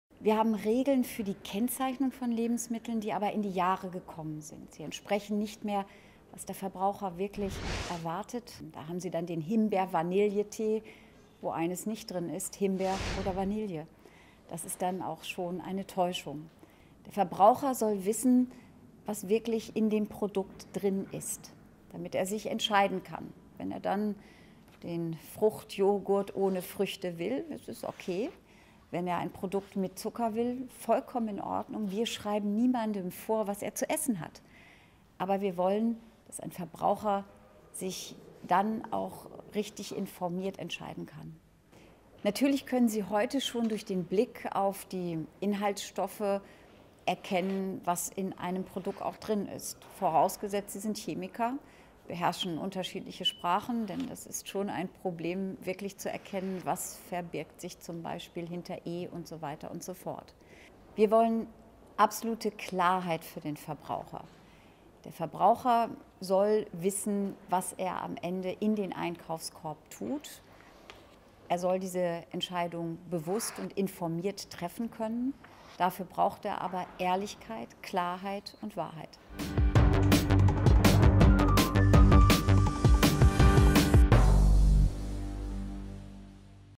Connemann: Was draufsteht, muss auch drin sein
Die stellvertretende Vorsitzende der CDU/CSU-Bundestagsfraktion, Gitta Connemann, erklärt im Video die anstehenden Reform der  Lebensmittel-Kennzeichnung in Deutschland.